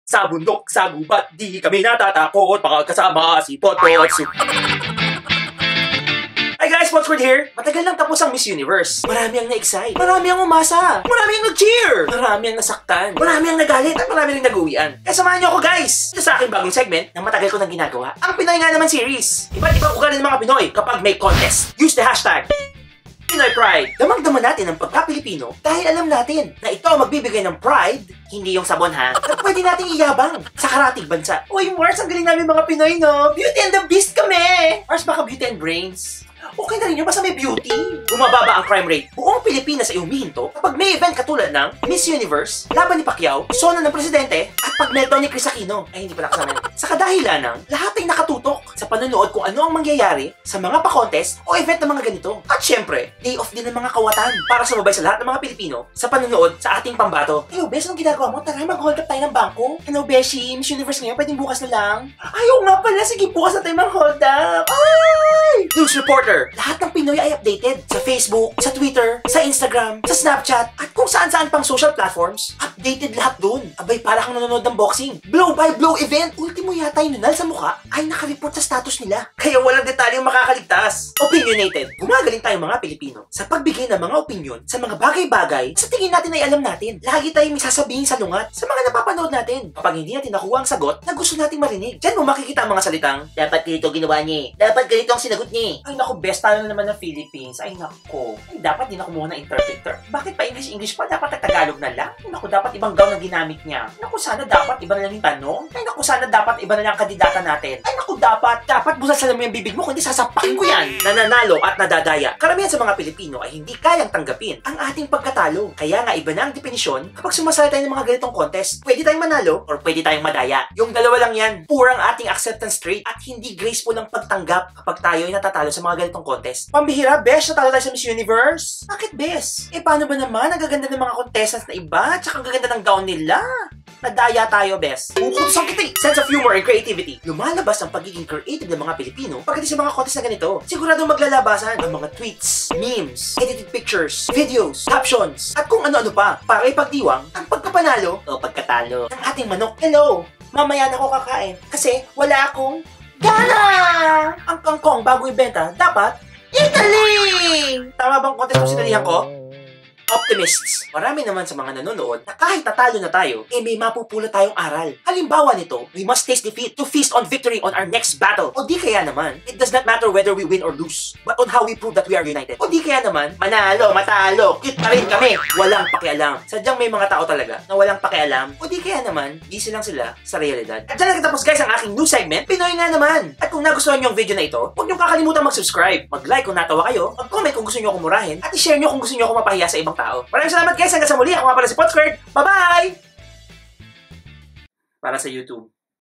PinoyNgaNaman : Iba't Ibang Ugali ng Pinoy Tuwing Nanonood Ng Contests | Tagalog | potsquared
PinoyNgaNaman a brand new old segment sa aking channel. Will discuss mostly Filipino traits that we are proud of or more likely be ashamed of. Pinoy nga naman!